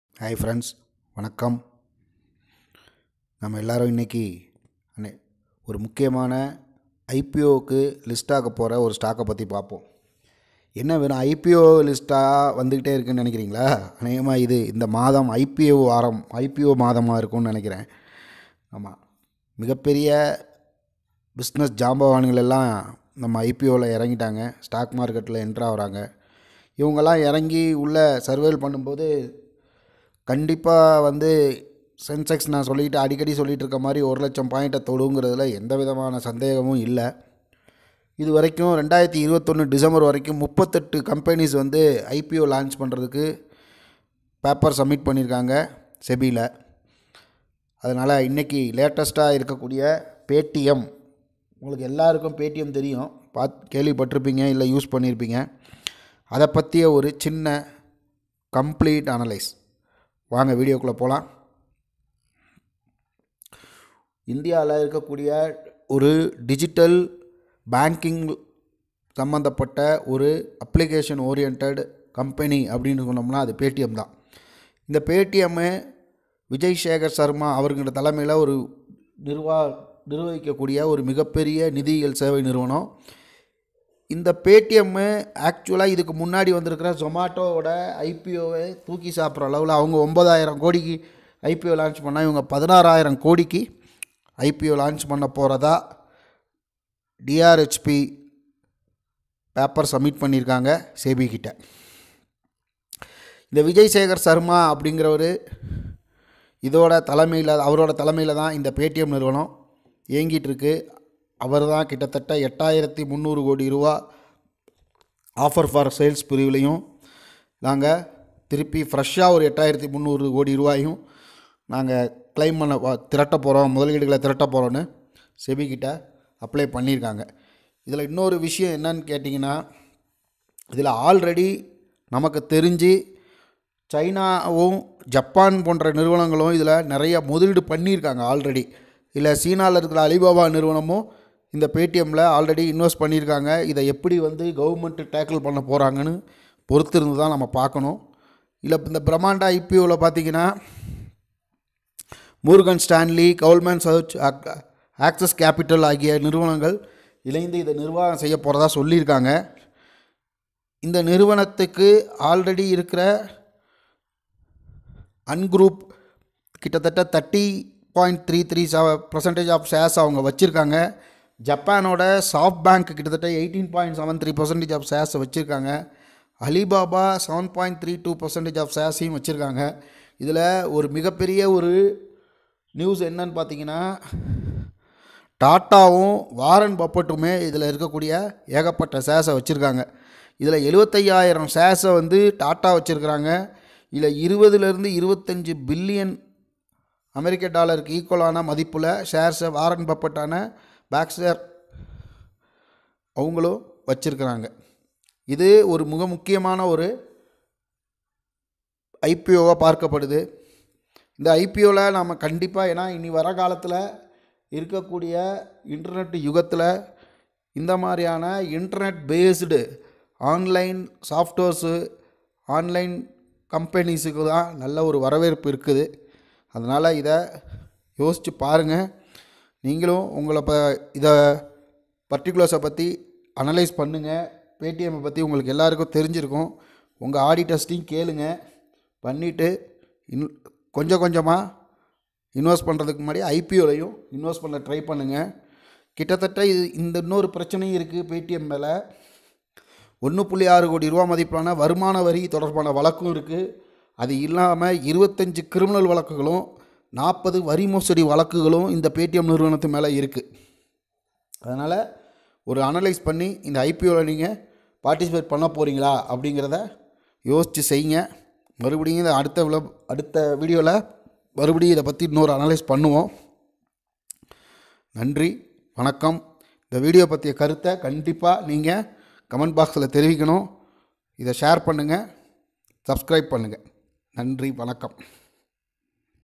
PAYTM IPO in tamil | 16000 Crore IPO | Paytm IPO Detailed Analysis | Biggest IPO in India |NSE IPO
PAYTM IPO in tamil  | 16000 Crore IPO  | Paytm IPO - Detailed Analysis | Biggest IPO in India
Tata Invested IPO , alibaba invested PAYTM